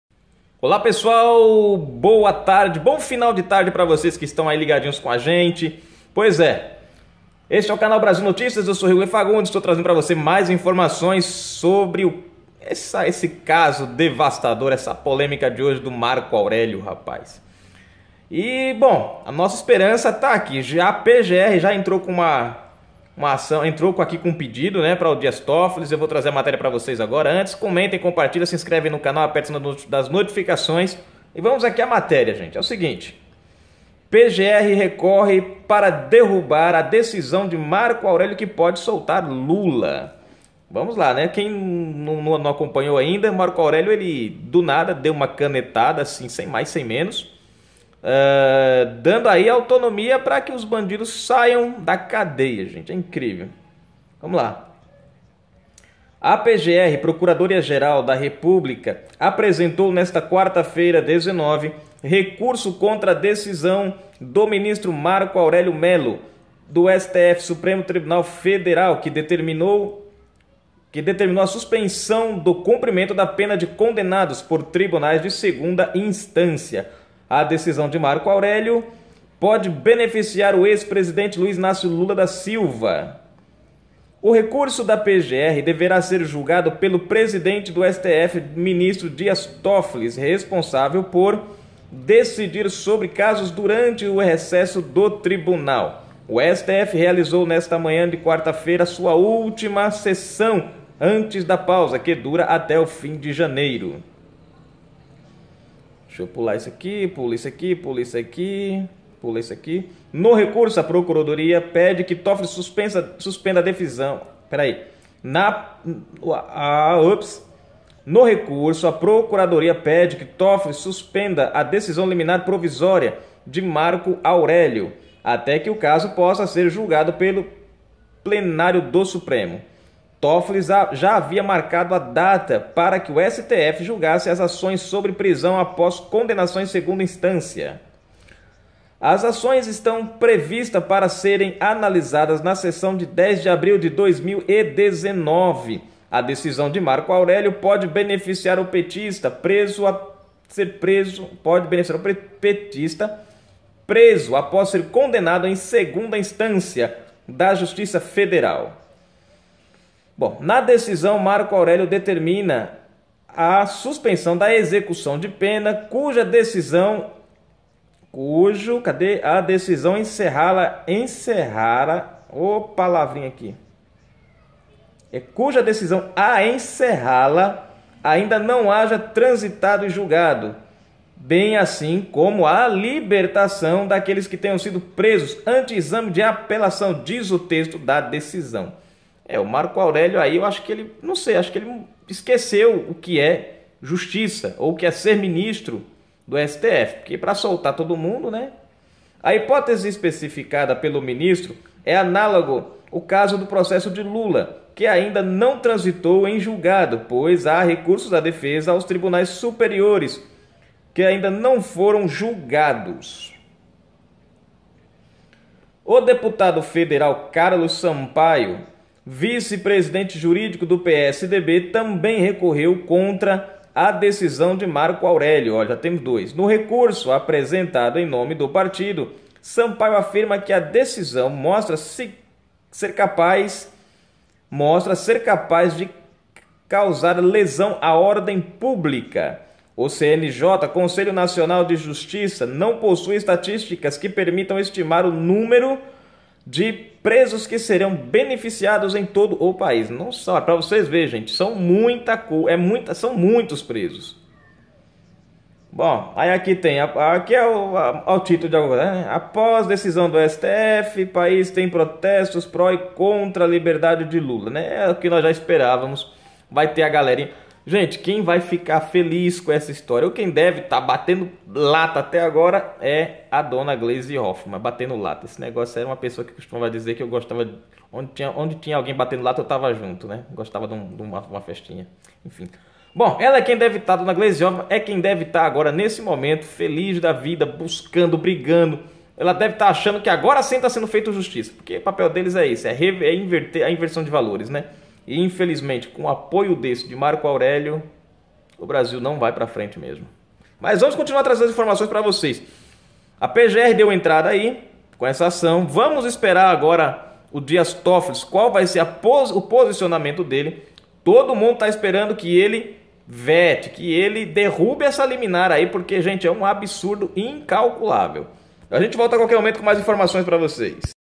PGR recorre para derrubar decisão de Marco Aurélio que pode viabilizar a soltura de Lula
A procuradoria geral da república, entrou com um pedido para derrubar a liminar de Marco Aurélio, cabe agora ao presidente do STF decidir se mantem ou derruba.

Canal Brasil Noticias trazendo a informação até você.